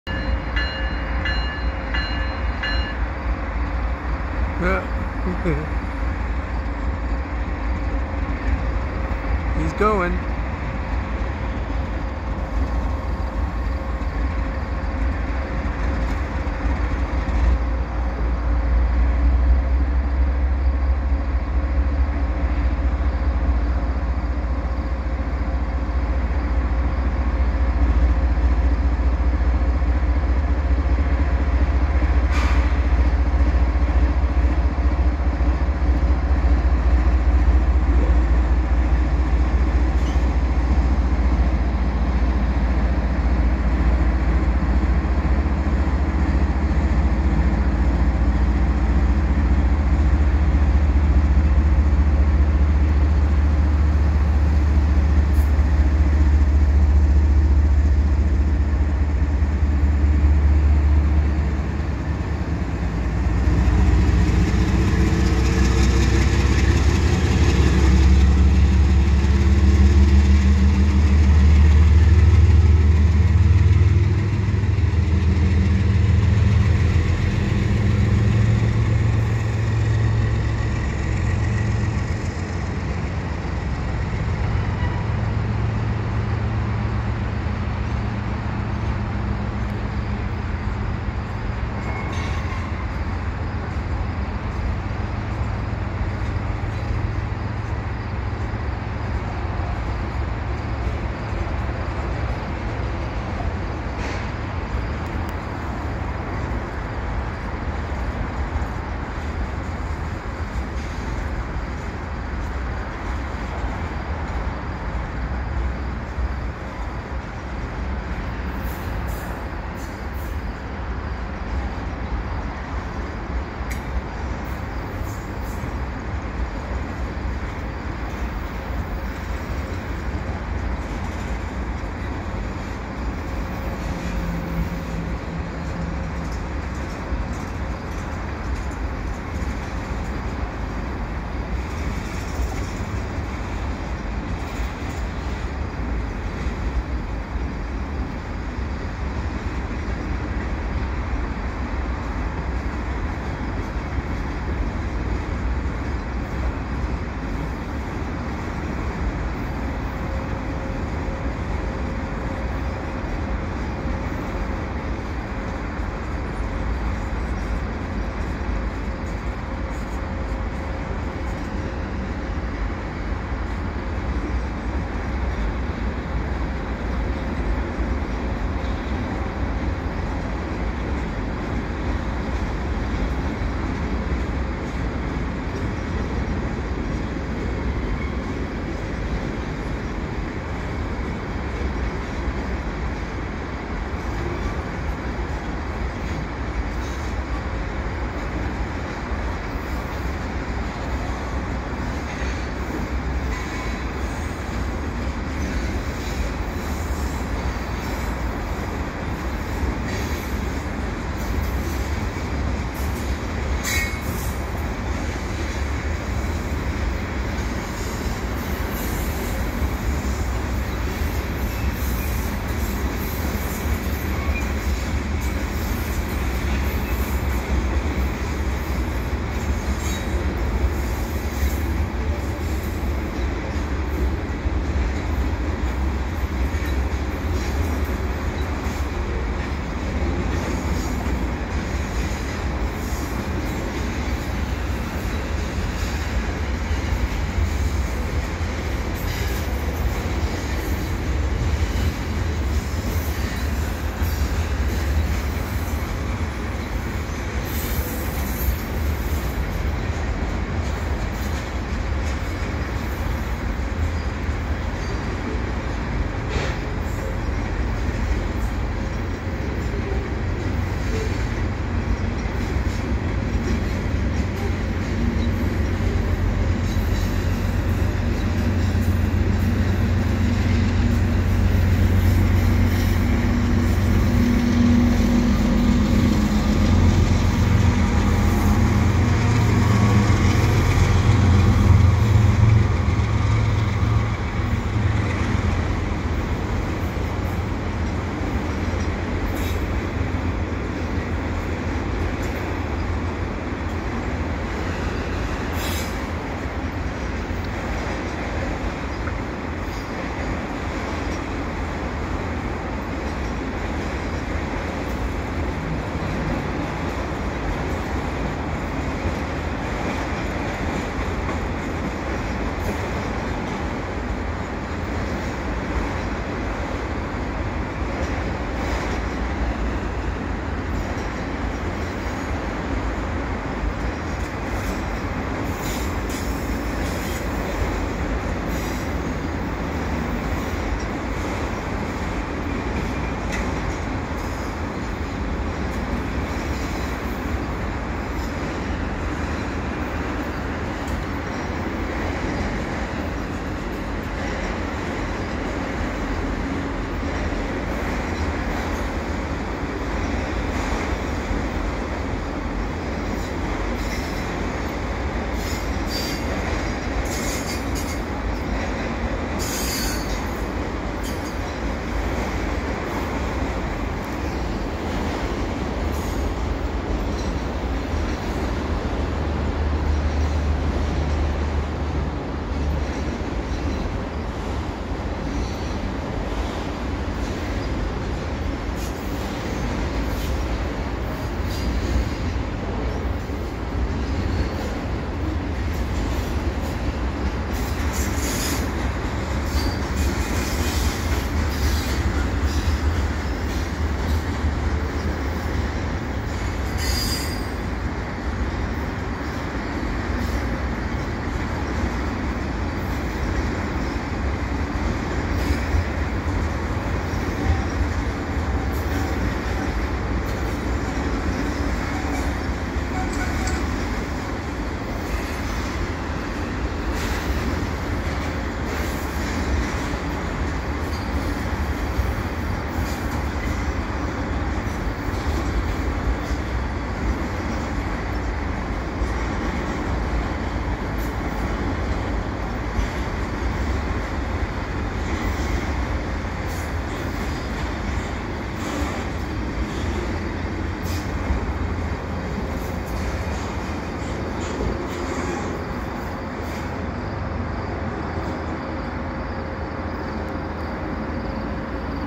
TRAINS CP Freight With TRE F59PH Departs Under Dorval Ramps
Finally, Westbound CP119 mixed freight and double stacks pulled by the usual CP GE ES44 and foreign TRE F59PH from the Dallas , TX Trinity Railway Express commuter agency departs Dorval underneath the airport ramps at dusk